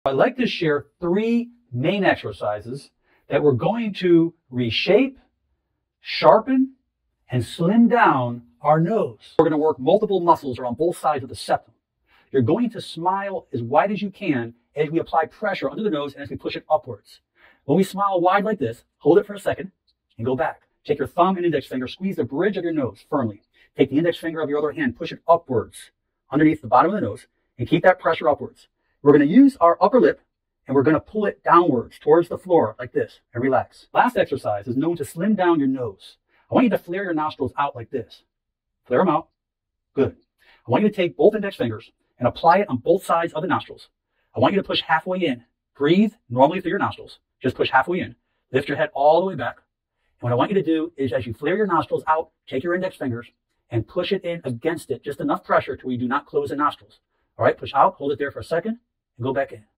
Reshape, Sharpen, & Slim Down Your Nose (Speeded Up) DR. MANDELL
Many people spend a lot on cosmetic surgeries to get that perfectly shaped nose. On the other hand, there are some who do not want to bear any kind of risks that these surgeries may involve. Well, for all those desiring a perfect nose at no cost, here are some exercises that can help you change the shape of your nose.